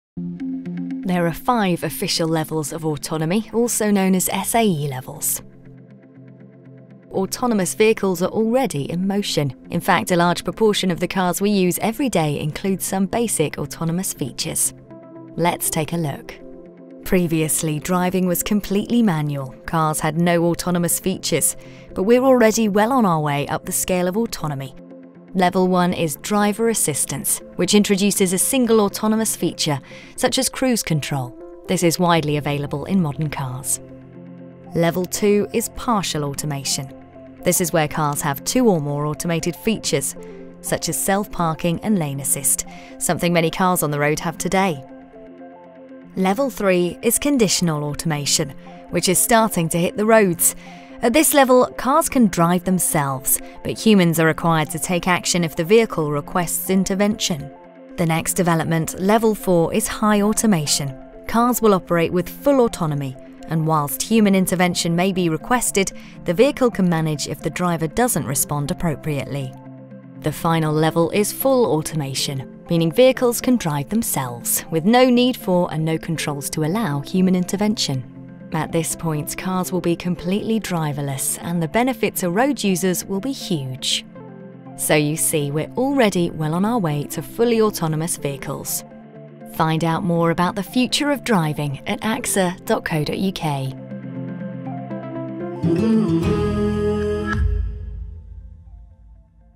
Driverless Cars | The 5 levels of Autonomy (SAE Levels) | AXA UK
Autonomous cars are already on our roads! This short animation explains the 5 different levels of vehicle autonomy and uses these to look forward at where the future of driving is heading.